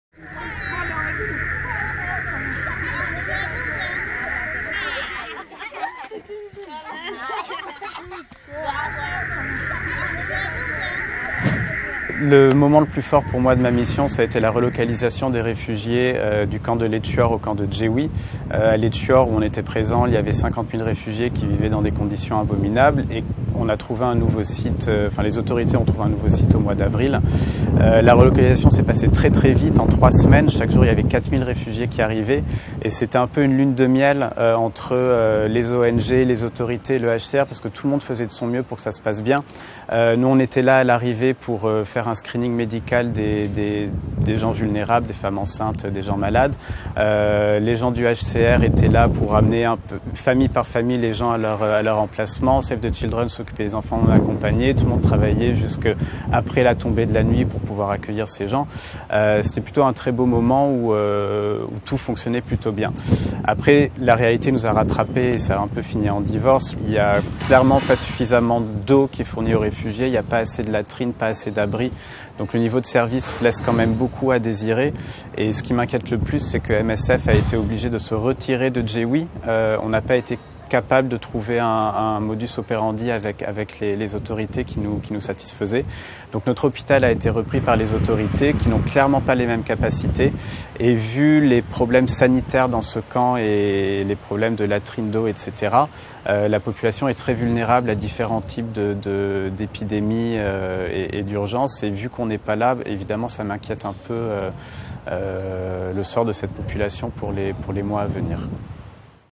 Relocated Refugees Still Vulnerable in Ethiopia
Until last month Doctors Without Borders/Médecins Sans Frontières (MSF) worked in Leitchour camp in Ethiopia, where refugees were seeking shelter from the conflict in South Sudan. The condition of the camps were appalling - some 50,000 people living in the worst conditions. Deputy Head of Mission Brice Garnier describes MSF's role in moving these refugees to a different location and the concerns the organization has now about the conditions in the new camp.